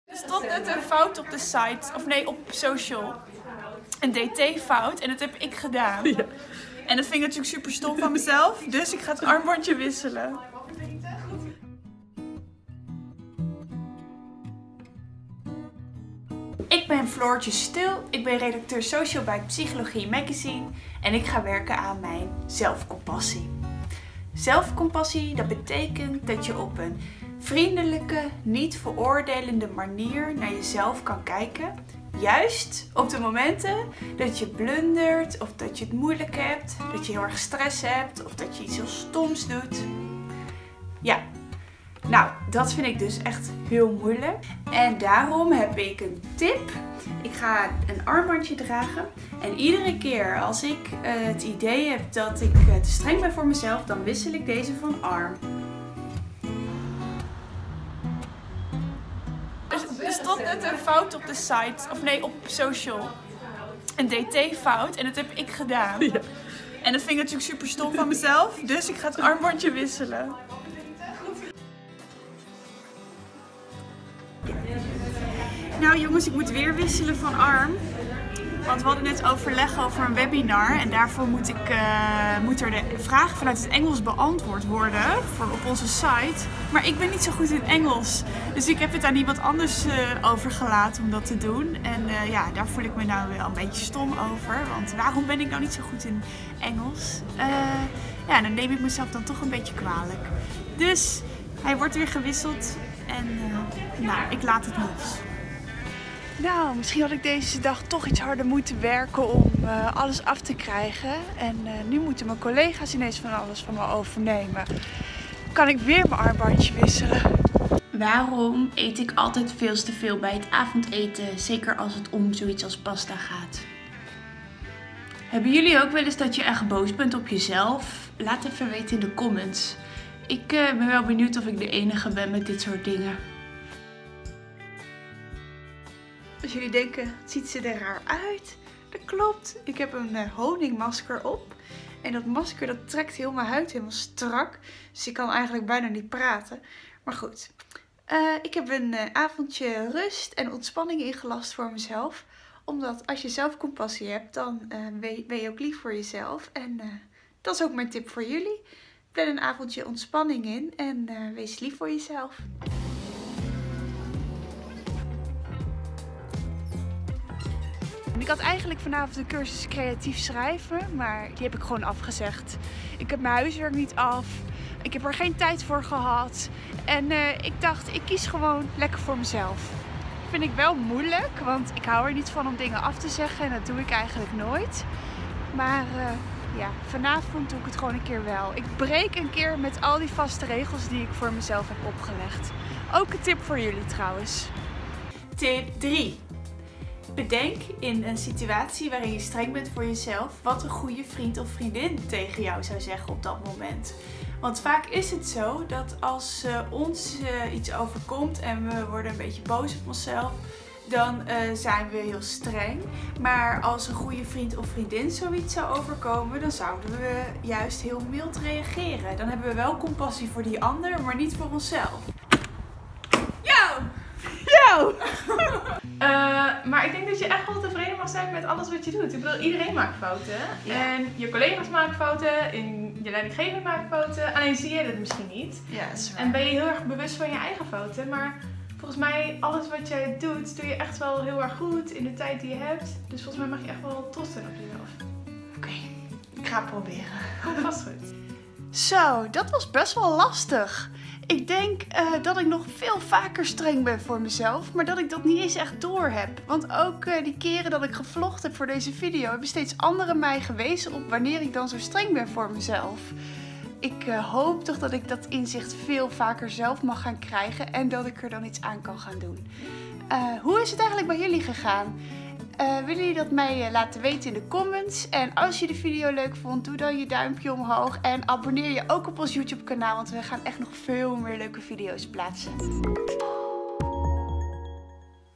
Hoe verminder je ZELFKRITIEK? | RUST IN JE LEVEN | Psychologie Magazine
Hoe vermindert redacteur Floortje haar zelfkritiek?
We moeten slim én succesvol én knap én interessant zijn. Lukt dat niet, dan zijn we niet mals voor onszelf. Maar al die zelfkritiek maakt het leven niet vrolijker. Zo word je milder voor jezelf.